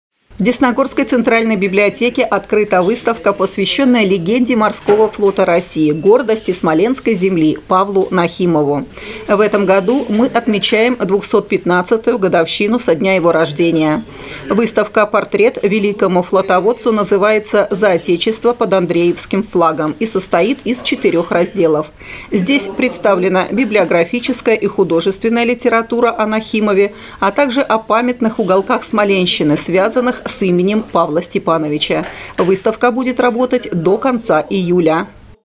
Десна-ТВ: Выставка-портрет, посвящённая адмиралу Нахимову
В Десногорской центральной библиотеке открыта выставка, посвящённая легенде морского флота России, гордости смоленской земли, Павлу Нахимову.